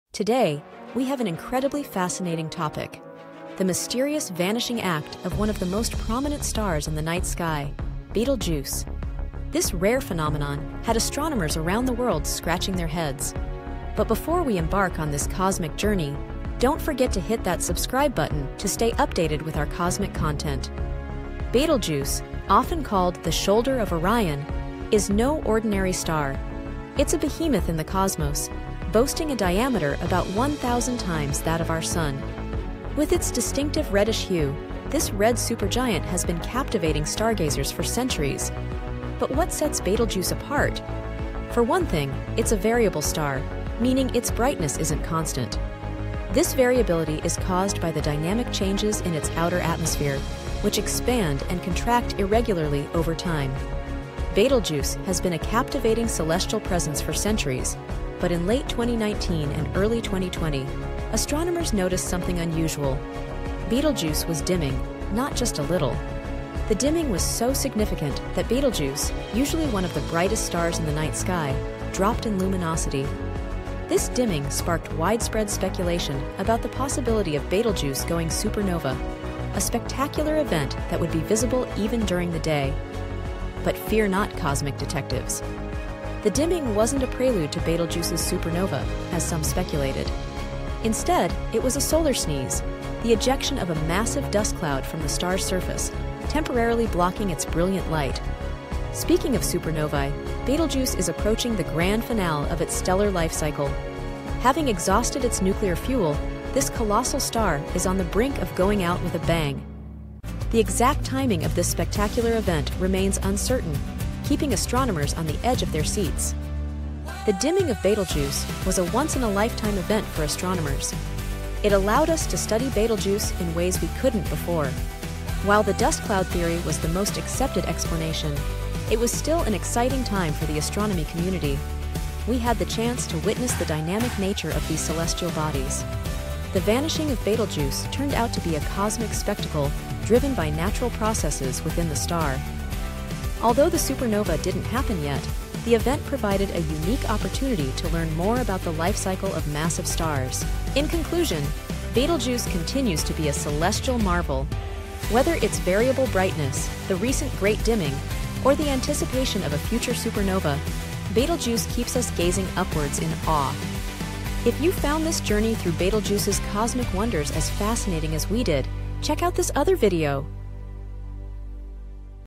When Betelgeuse Went Dark: The Universe's Greatest Mystery
Unraveling Betelgeuse's Celestial Secrets! 🌠 Join us on a cosmic journey as we delve into the intriguing phenomenon of Betelgeuse's mysterious dimming. In this video, we unlock the cosmic enigma, exploring the breathtaking light show that left astronomers astounded. From the star's epic vanishing act to the cosmic drama of its temporary eclipse, we decode the celestial mysteries surrounding Betelgeuse. Brace yourselves for a stellar adventure as we navigate through the twinkling wonders of the universe and reveal the secrets hidden within the heart of this colossal red supergiant. Don't miss out on this cosmic spectacle – hit play and join us as we journey 'Beyond the Stars'!" ✨🚀

Week In Review: PJAYICLWRYK7Z99U
Just Look Up: E4FC83L8KKWTVJGD